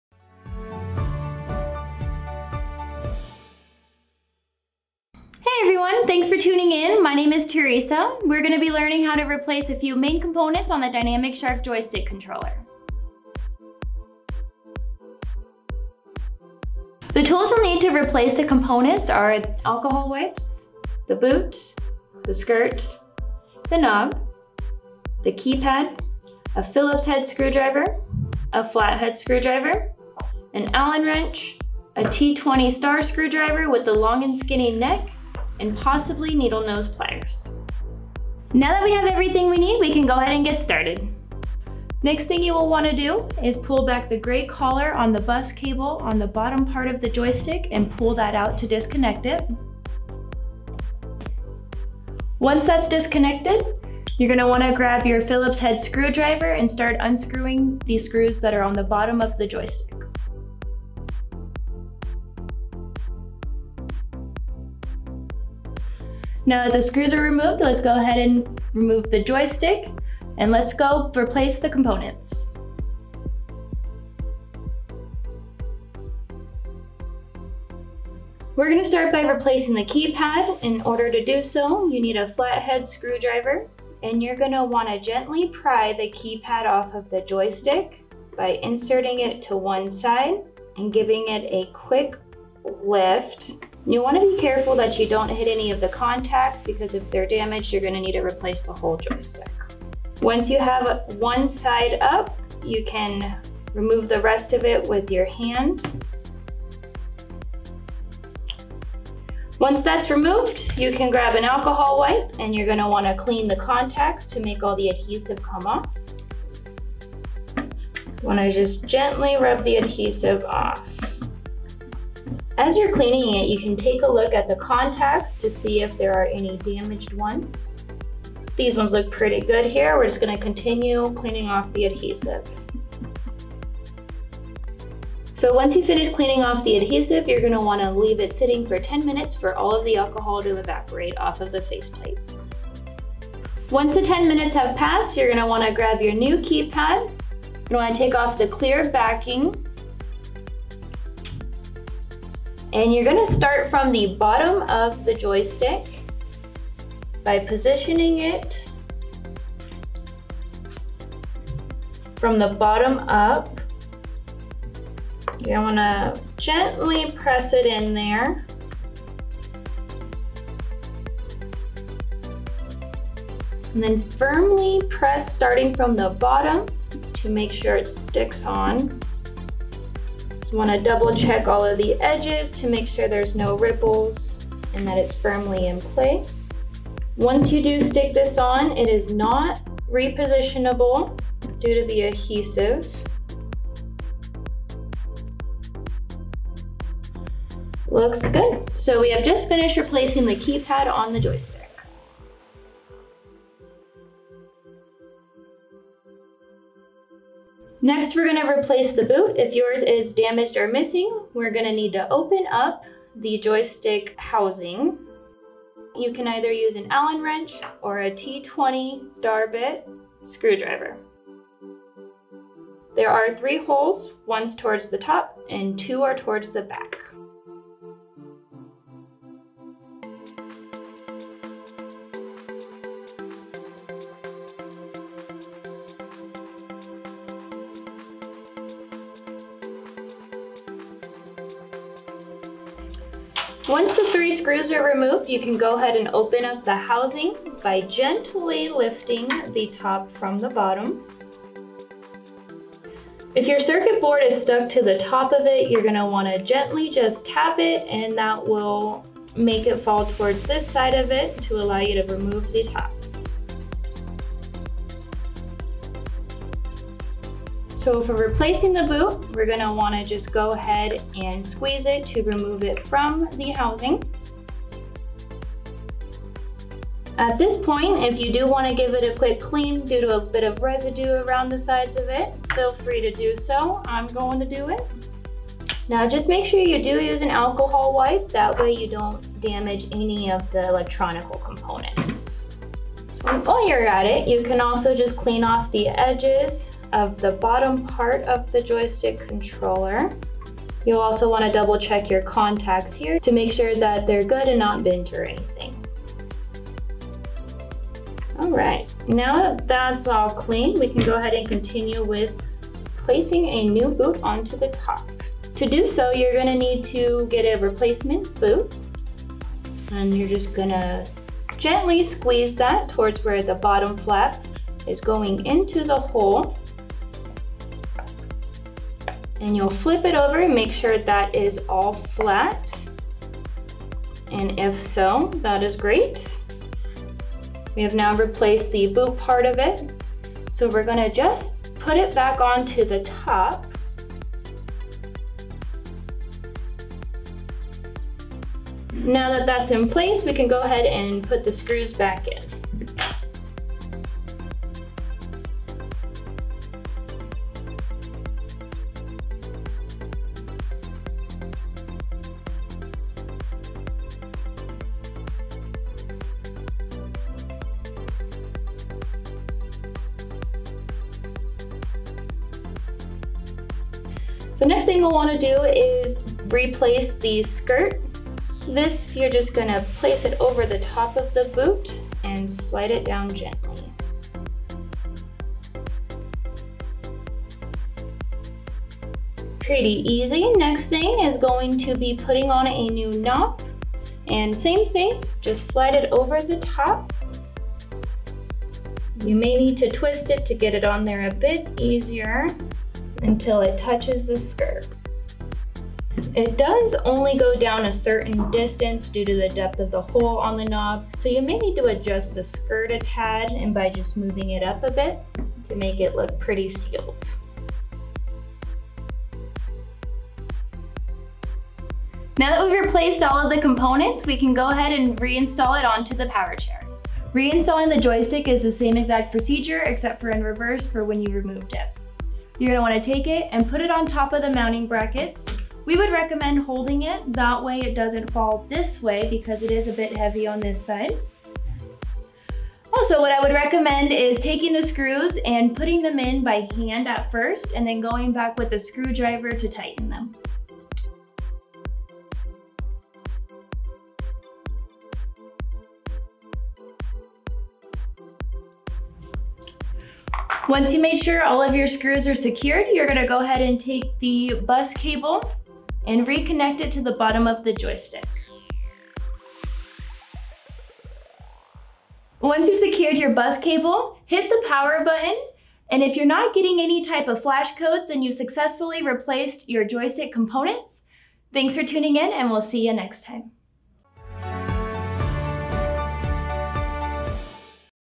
How to Replace the Keypad, Knob, Skirt, and Boot on a Dynamic Shark Joystick
If your joystick's keypad is no longer as responsive as it once was, it may be time to replace it and restore functionality. And if your joystick is functioning perfectly fine but has seen better days looks-wise, perhaps it would benefit from a revitalization by replacing some of the components.

In this video, we'll show you how to replace a Dynamic Shark Joystick Keypad, Knob, Skirt, and Boot so you can restore your joystick's functionality and keep moving.

Don't need to know how to replace all of the components?
Skip to 1:12 to see how to replace the keypad
Skip to 3:21 to see how to replace the boot
Skip to 6:08 to see how to replace the skirt & knob

Let Monster Scooter Parts show you how simple it can be to replace some of your Dynamic Shark Joystick components!